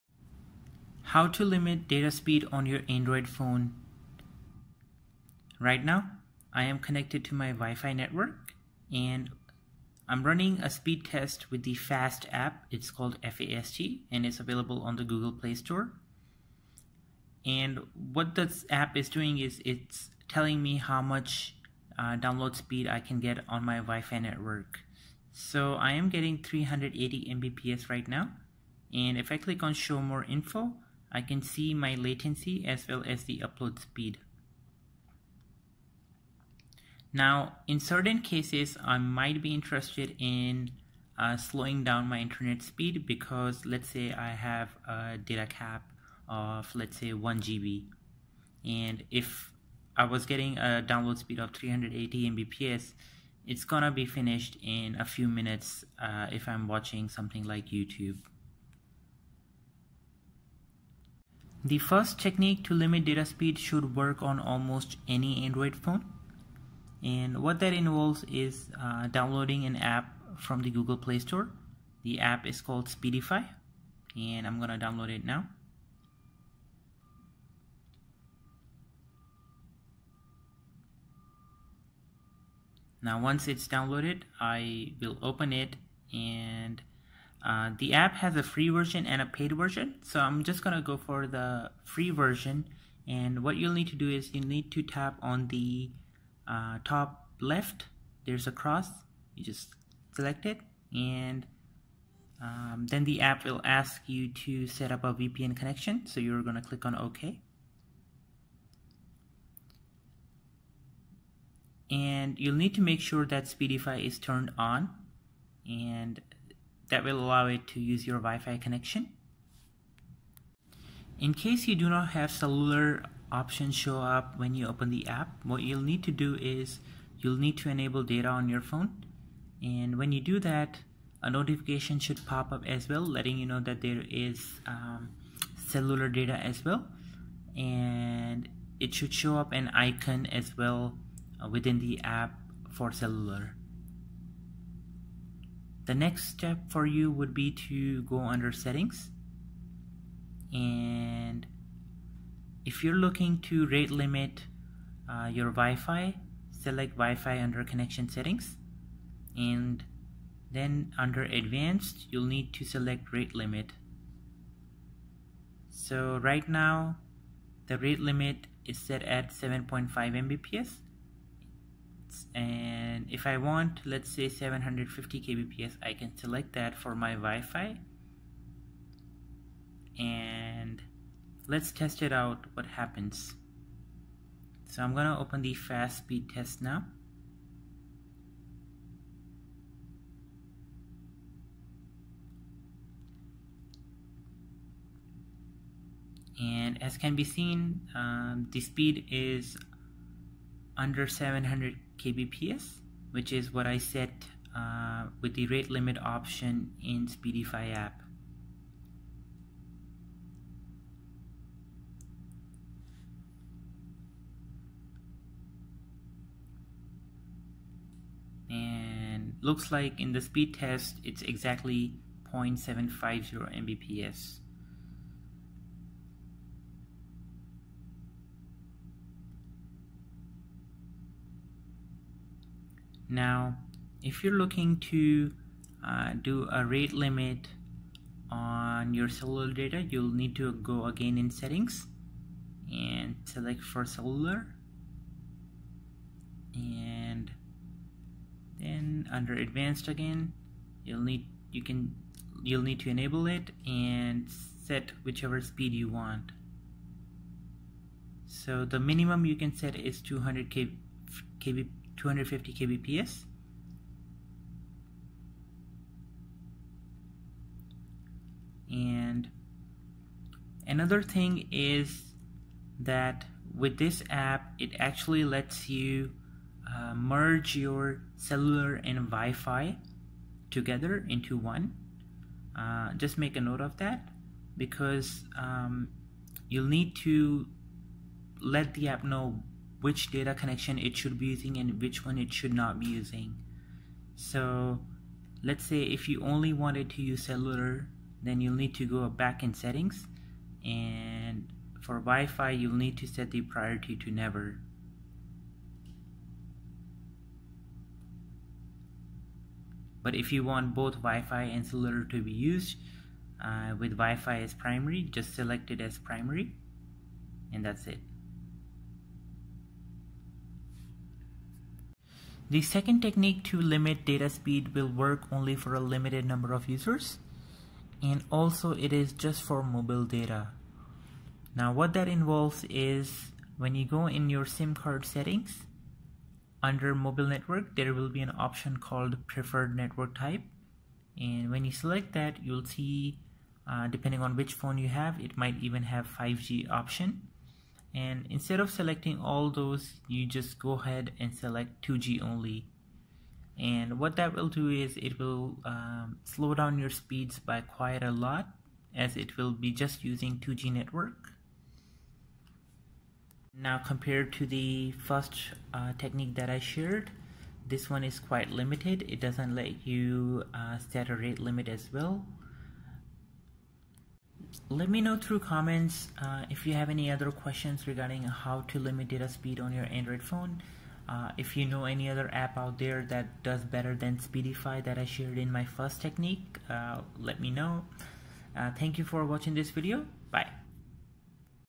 How To Control Mobile Data Speed on Android? - Works For both Mobile Data And WiFi! - Android App!
Tutorial on how you can limit internet data speed on your android phones. You might be interested in limiting download speed on your phones because maybe you are testing something or you have data caps with your internet service provider.

While this functionality has been a lot in demand, there are rarely any apps that perform this functionality so I took some time to find one that works the best! A lot of the apps out there focus on limiting data usage which is a feature that is already inbuilt on most new android phones.

This tutorial should work on most android phones and you do not need to root your phone either (works on non-rooted phones). Also completing this tutorial you would be able to rate limit data speeds on your WiFi as well as Mobile Data (3G/4G/5G).

This tutorial has been demonstrated using app - Speedify available for free on Google Play Store.

I think this functionality should have been provided within the developer options on the phone.